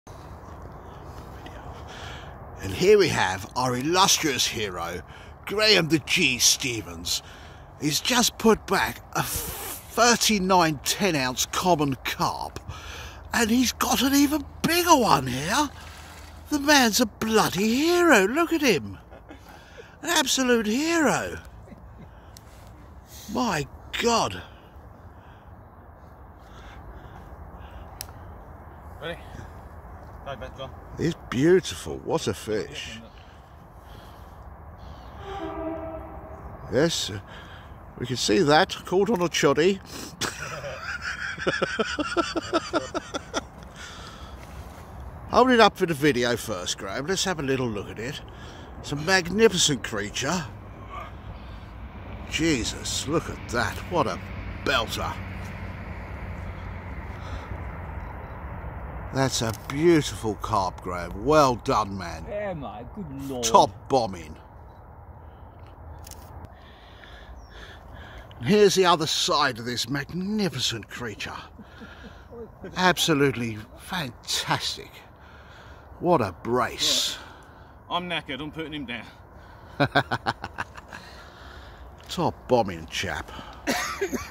Graham Stevens with Nigel's Common Carp 42-12 from Frimley Pit 4. Wraysbury Baits. JPrecision Hooks.
Graham Stevens with Nigel's Common Carp at 42-12 from Frimley Pit 4. Narration and video by John Merritt.